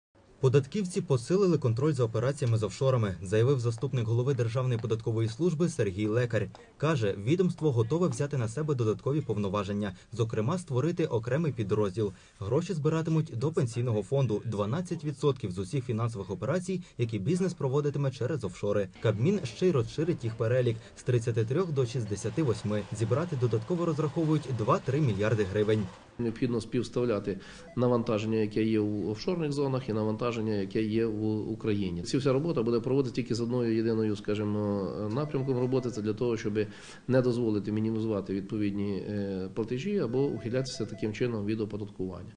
Податківці посилили контроль за операціями з офшорами
ubr.ua - украинский бизнес ресурс : Податківці посилили контроль за операціями з офшорами. Заявив заступник голови Державної податкової служби Сергій Лекарь.
Каже: відомство готове взяти на себе додаткові повноваження. Зокрема, створити окремий підрозділ . Гроші збиратимуть до Пенсійного фонду. 12 % з усіх фінансових операцій, які бізнес проводитиме через офшори. Кабмін щей розширить їх перелік - з 33 до 68-ми. Зібрати додатково розраховують два-три мільярди гривень.
Сергій Лекарь, заступник голови ДПС: &quot;Необхідно співставляти ті навантаження, які є в офшорних зонах і навантаження які є в Україні. Ця вся робота буде проводитись тільки з одною метою - це для того, щоб не дозволити мінімізувати відповідні платежі або ухилятися таким чином від оподаткування&quot;. .